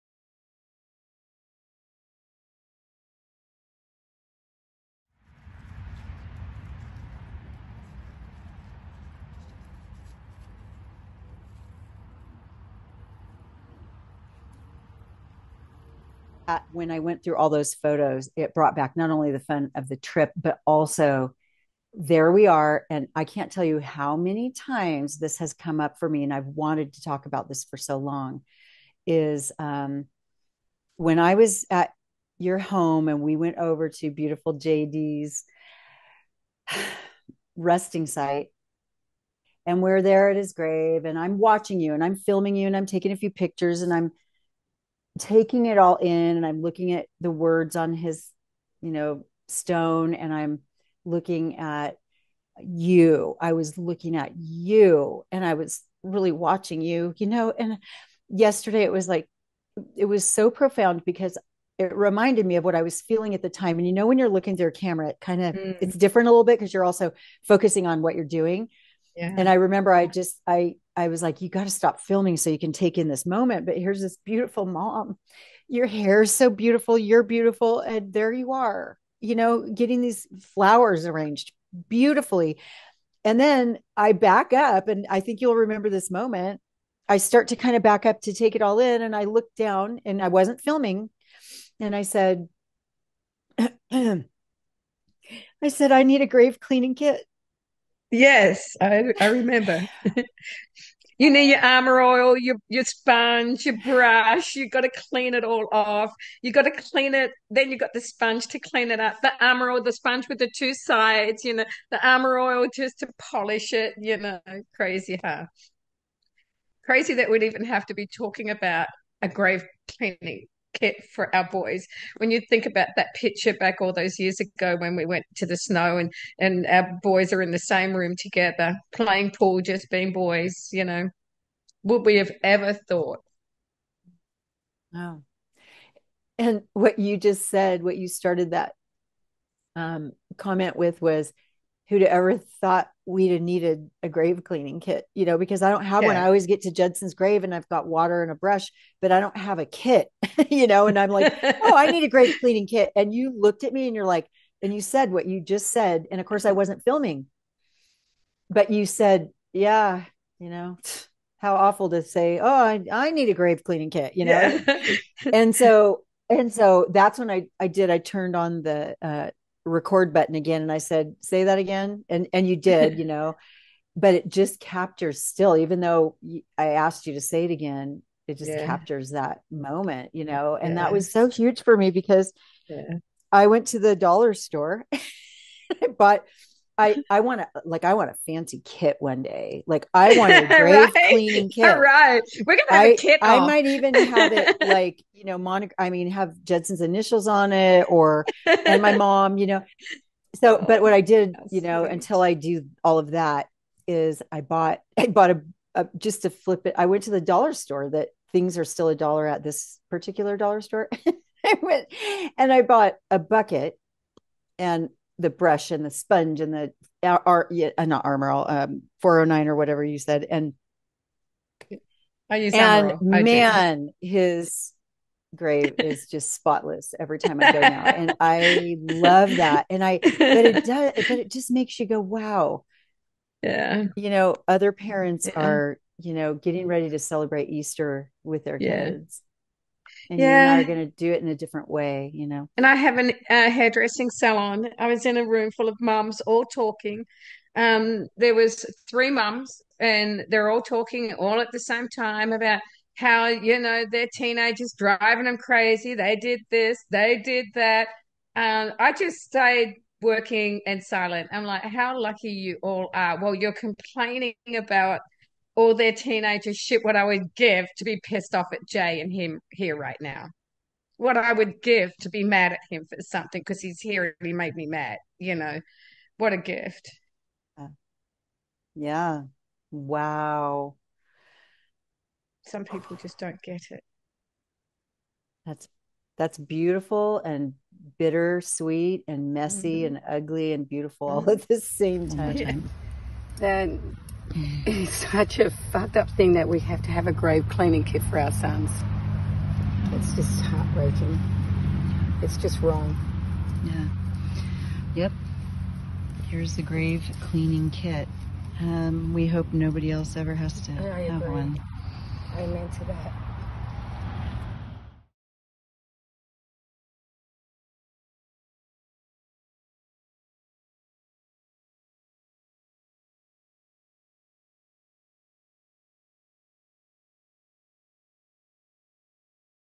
"I need a Grave Cleaning Kit too..."
The story behind this episode of "Angel Moms Talking" is one of those, "...you had to be there moments. I traveled to Ione to visit my lovely friend Robyn. Her incredible son, JayD, died tragically in the war in 2007. He was and IS, a hero.

Meanwhile, we made our way over to his grave. Robyn promptly grabbed some items out of the back of her car and we walked over to JayD's grave. And I started to film this beautiful woman cleaning and scrubbing JayD's grave marker, and then she started to arrange flowers, trimming and placing them one by one, just right.

And I backed off, stopped filming and watched this happen, sinking into the moment, holding in reverie the event unfolding in front of me, and I suddenly blurted out, "I need a grave cleaning kit too!"  WTH? I could not believe those words fell out of my mouth, and Robyn said, "I can't believe we have to have grave cleaning kits for our beautiful boys..."

Here, we talk about the experience, and once again, unapologetically take it all in, absorb the gravity of this soul-sucking experience of child loss.

xoxo,
Jami and Robyn - Angel Moms,
and our precious Angels, Judson and Jay'D

*Warning, adult language, listen with care.